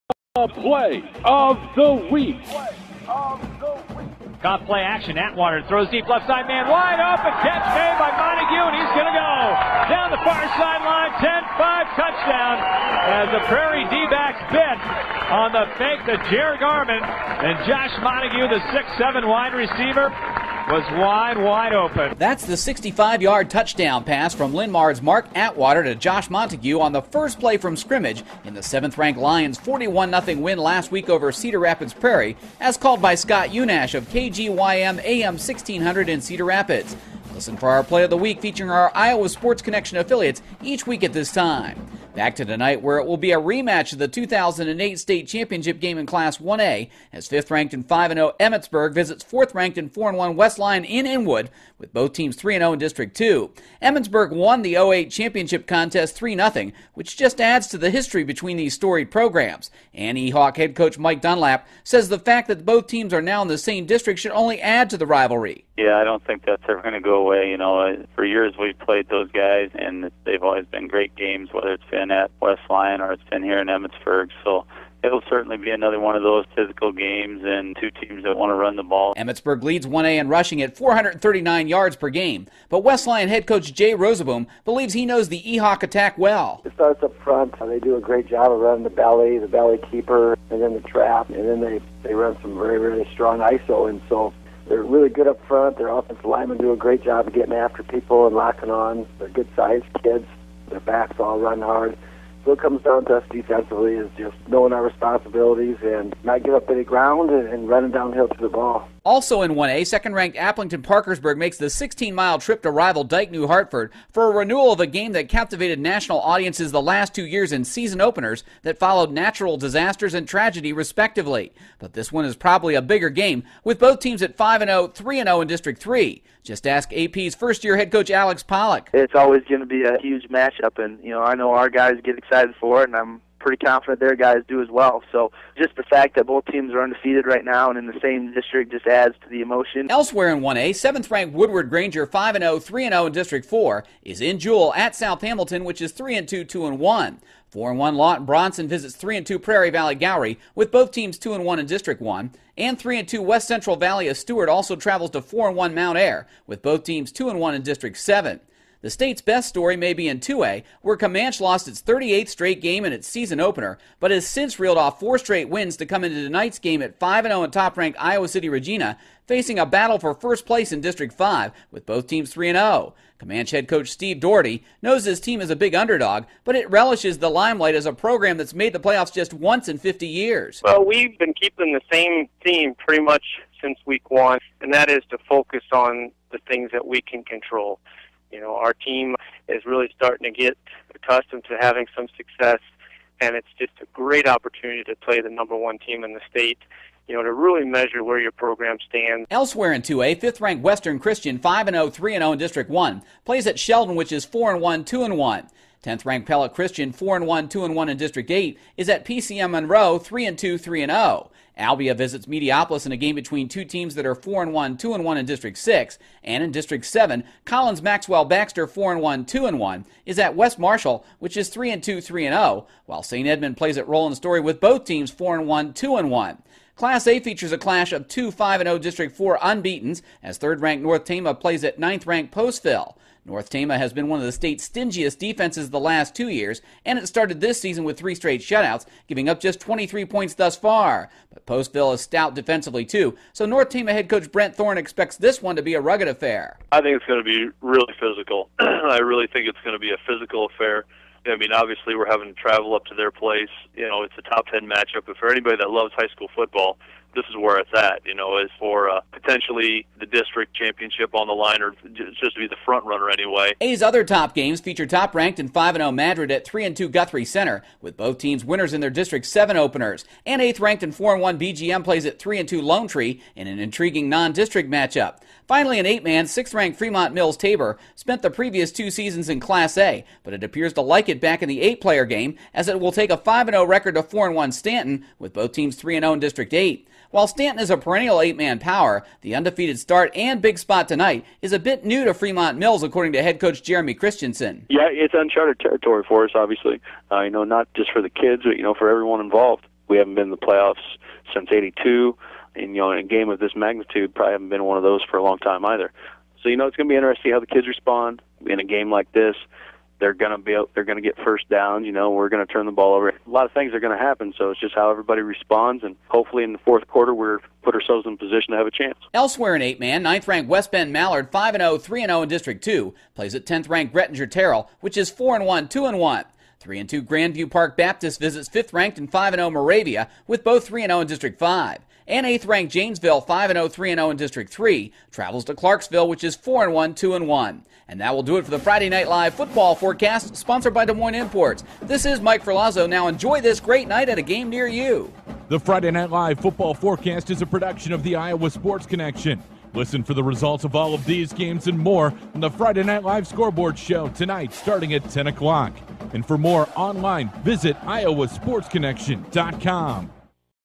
Mike Ferlazzo's Friday Night Forecast (10/01/2010)
Mike Ferlazzo breaks down all the big week 6 action.